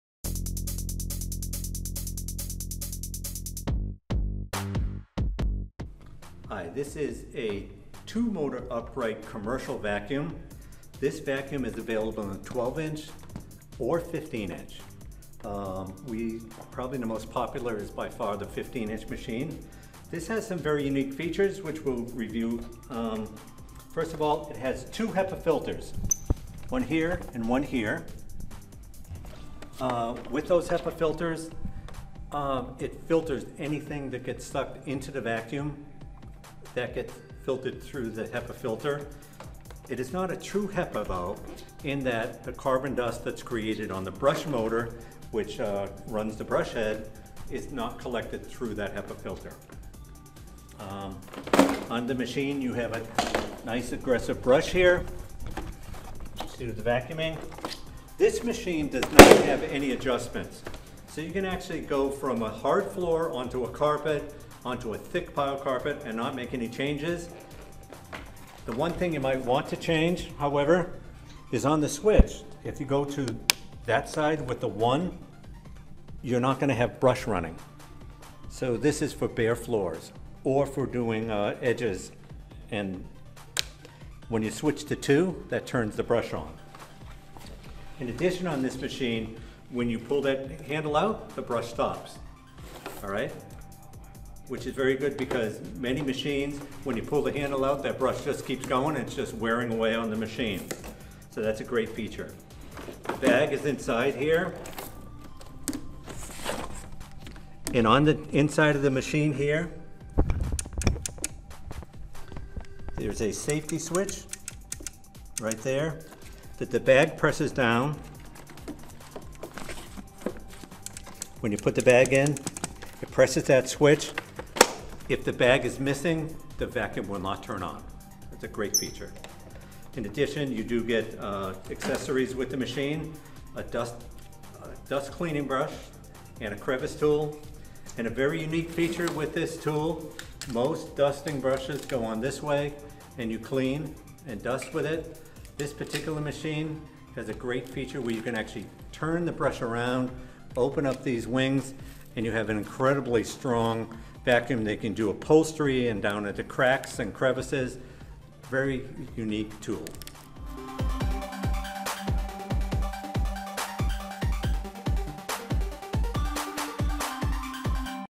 ProTeam Proforce 1500XP Commercial Upright Vacuum Review
The ProTeam® ProForce 1500XP is a high-performance, dual-motor vacuum that is extremely reliable. It offers powerful soil removal on both hard and soft surfaces, and can be equipped with various tools for edge and detail cleaning.

Read more to find out what makes this one of the best commercial vacuums of 2021.